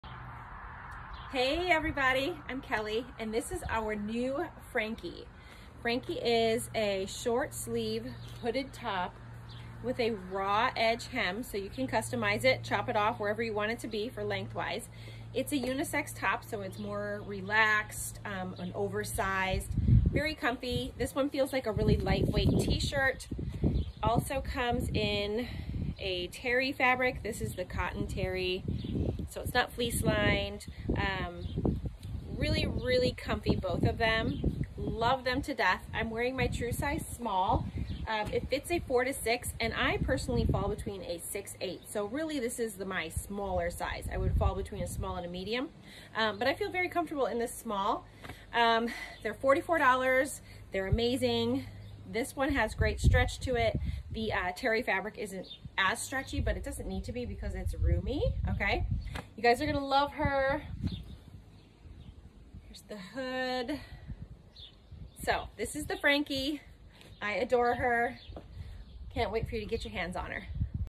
LuLaRoe Frankie Fit & Sizing Review
I'll be honest - I wasn't sure if I'd love Frankie or not. but of course I do. I'm loving the relaxed fit. It's the ultimate casual top. I love Frankie paired with our Rise collection, biker shorts, and denim the most. My true size falls between a small and medium. I felt comfortable in both sizes. Just for kicks I tried on an XXS and actually that fit great too. There's a lot of flexibility with sizing since it's a unisex style and a loose fit.

$44
XXS-00
XS - 0-2
S - 4-6
M - 8-10
L - 12-14
XL - 16-18
2XL - 20-22
3XL - 24-26

The details: This oversized short-sleeved hoodie is the perfect go-to whether you're chilling, lounging, or grabbing some lunch with friends. Our Frankie Short-Sleeved Hoodie is available in a variety of solids and prints that make it just right for layering or wearing alone, with raw edges that you can tailor yourself to get the perfect fit. The oversized fit makes it perfect for all body types.

Fabric: MVS - 95% Poly / 5% Span , CVC Terry - 63% Cotton / 37% Terry, and Poly Rayon Terry - 65% Poly / 32% Rayon / 3% Span. Sizes: XXS-3XL.